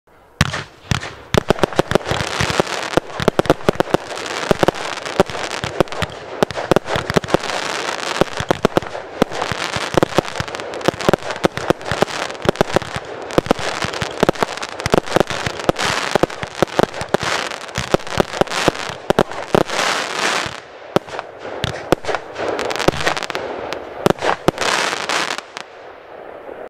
Experiments 4 x Russian Roulette
Simply a video of us multi firing a few cakes.